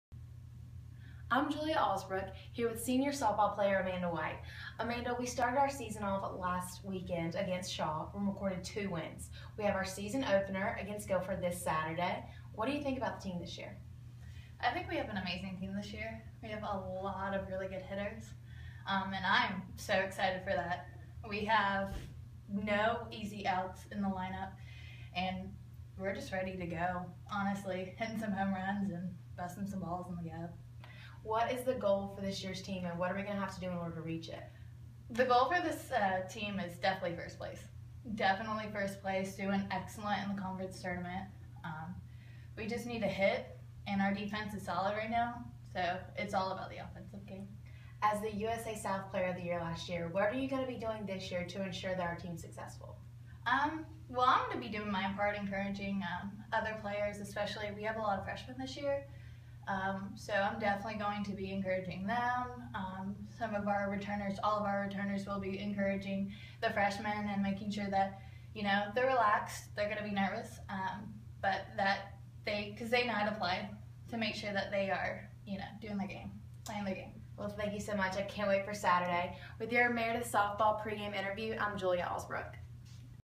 Softball pregame interview with Amanda White
Julia Allsbrook interviews teammate Amanda White before Saturday's home softball doubleheader versus Guilford. White is the reigning USA South Player of the Year. The first pitch is scheduled for 1 p.m.